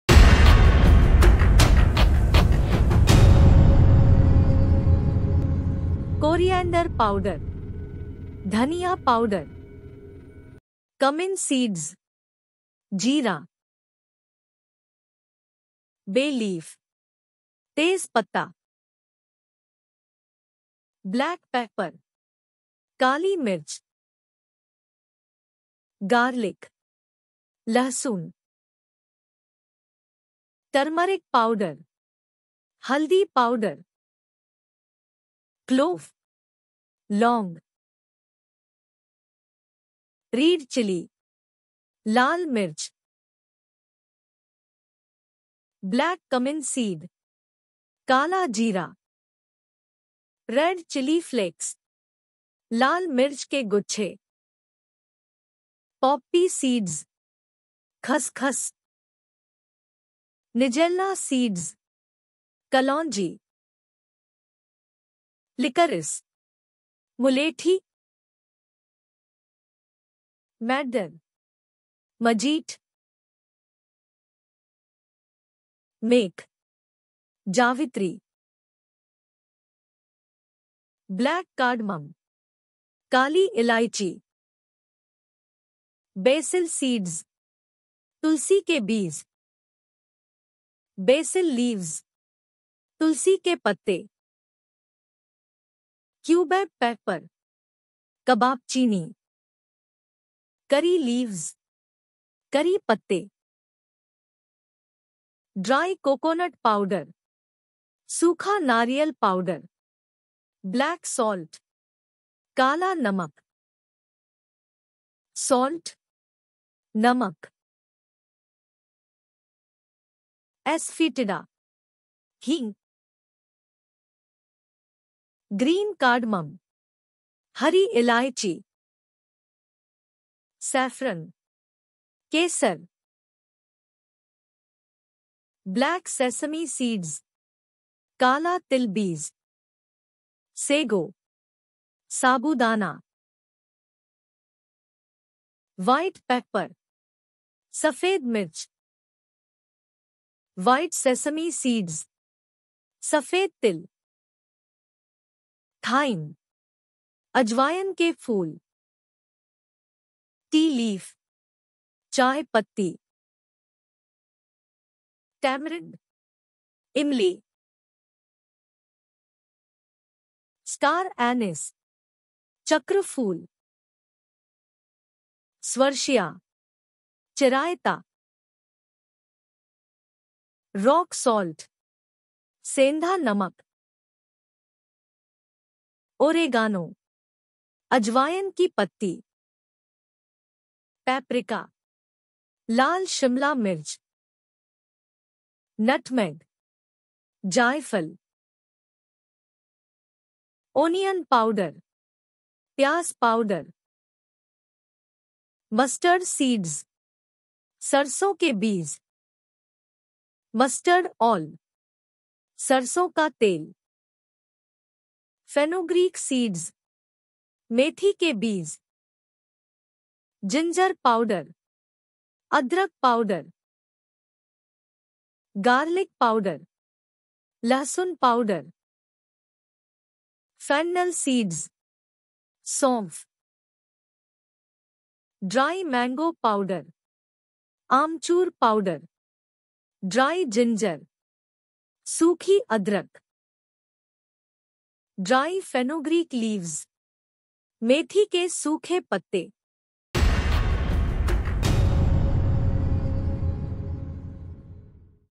50 मसालों के नाम हिंदी और अंग्रेजी में | Name of 50 Spices in हिंदी English @babyblackboard​
प्रिय मित्रों,
जानिए भारतीय मसालों के बारे में उनके अंग्रेजी और हिंदी नाम से। मैंने इस वीडियो में 50 मसालों के नाम उनके चित्रों और नाम के उच्चारण के साथ जोड़े हैं, जो आपको मसालों के बारे में सरल तरीके से जानने में मदद करेंगे।
Dear friends,
Know about Indian spices with their English and Hindi Name. I have added 50 spices name with their images and name pronunciation in this video, which will help you to learn about the spices in simplest way.